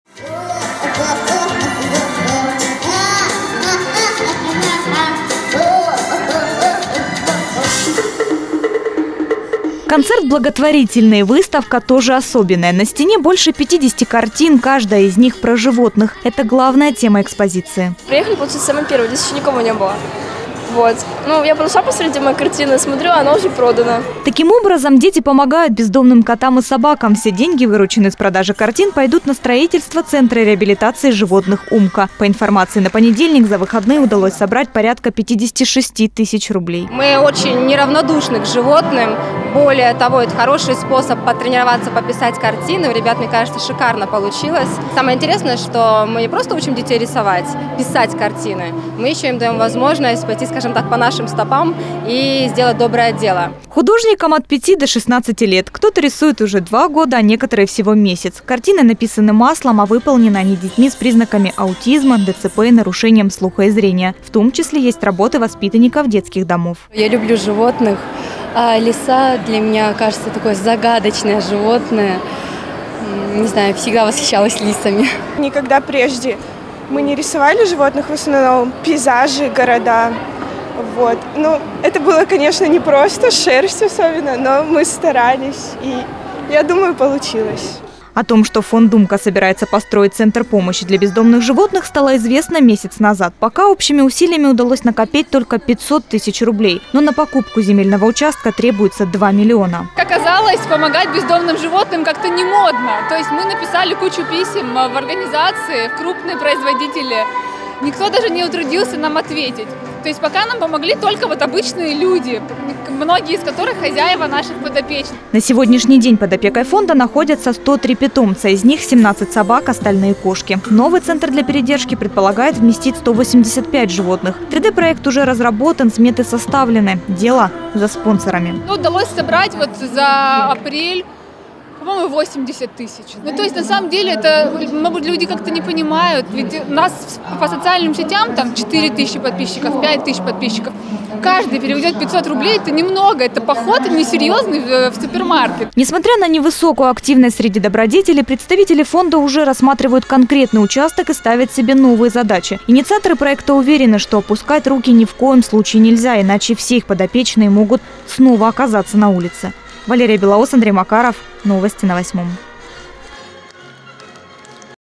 Центр помощи для бездомных животных собираются построить во Владивостоке
Благотворительная выставка-продажа картин прошла во Владивостоке. Дети написали картины в поддержку фонда помощи бездомных животных «Умка».